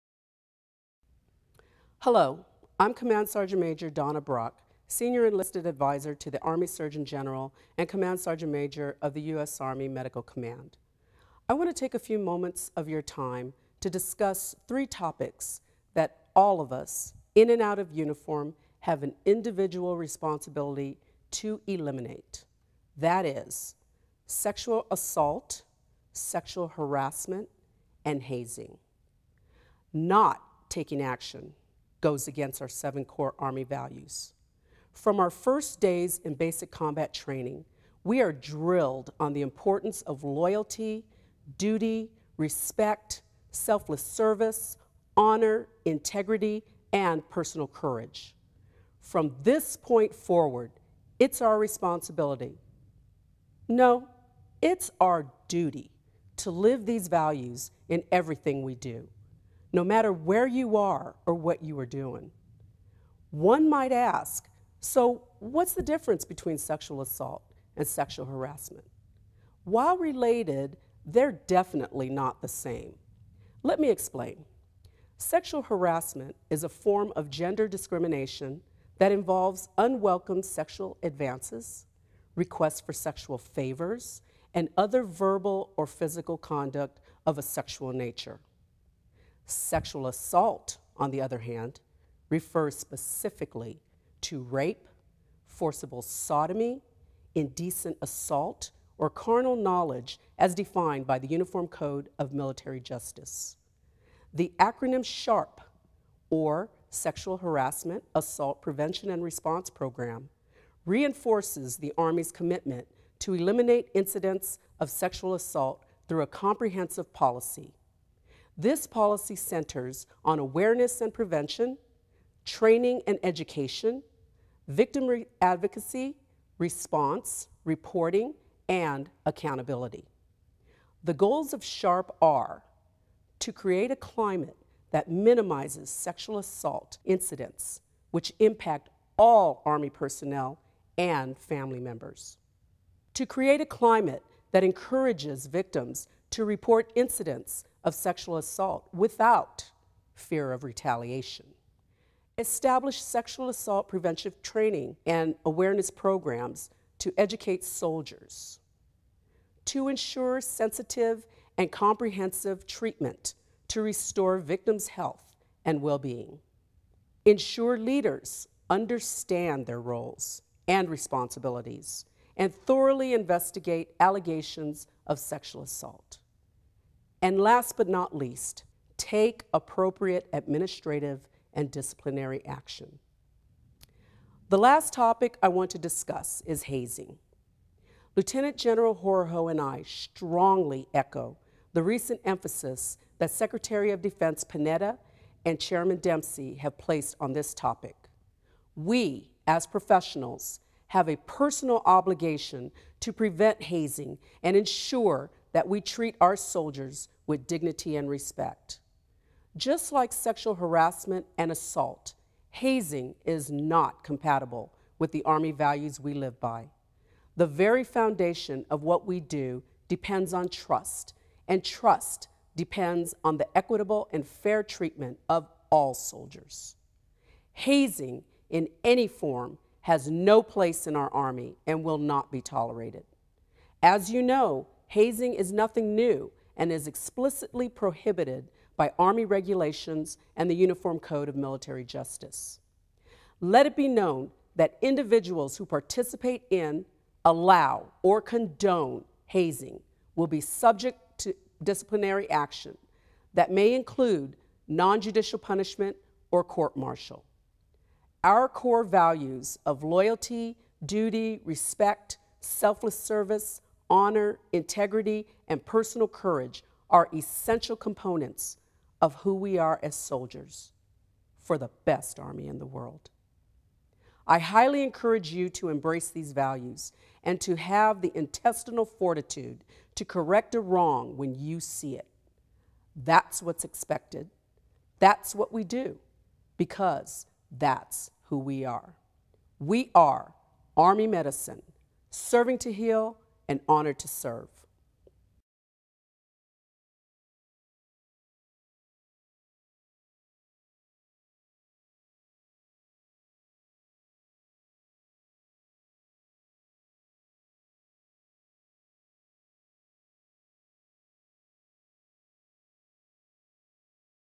SHARP Program & Hazing Video by CSM Donna Brock
U.S. Army Sexual Harassment Assault Response and Prevention Program and Hazing video by CSM Donna Brock, MEDCOM CSM